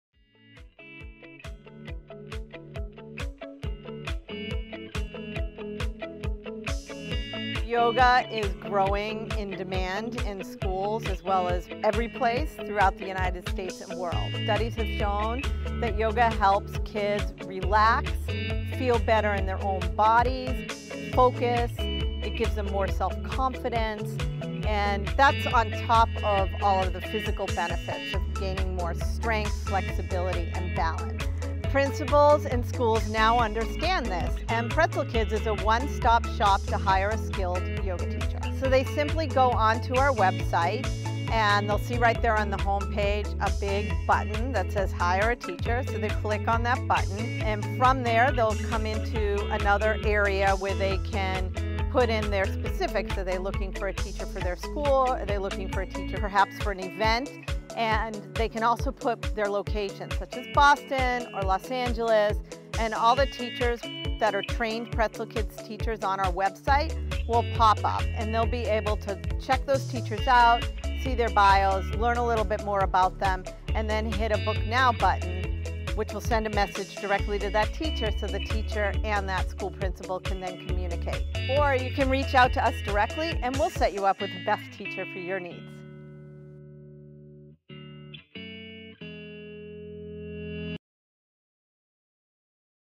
How to Hire a Pretzel Kids Yoga teacher
Want to offer Pretzel Kids yoga classes in your preschool, daycare, afterschool program, camp or other facility? Pretzel Kids yoga will come to you! Our trained and skilled kids yoga teachers come to your school with themed lesson plans that include: Pretzel Kids yoga games, mindfulness activities for kids, and more! Contact Pretzel Kids yoga today!